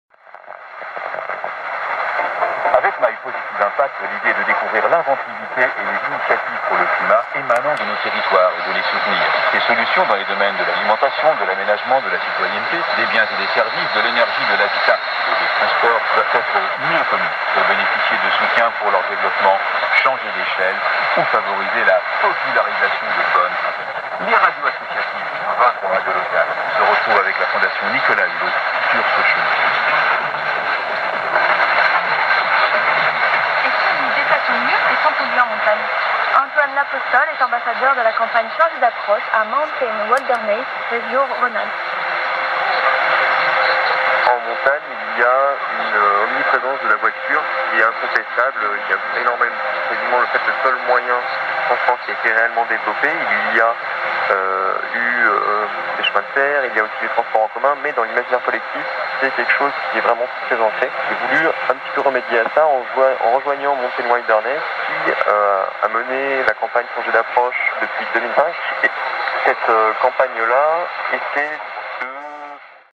1593 khz Bretagne 5 test transmission
28/06/2015  21:00 UTC
1593 Khz Bretagne 5 received in Germany
Tecsun PL-660 build- in antenna
Official start of this new station will be on 31/08/2015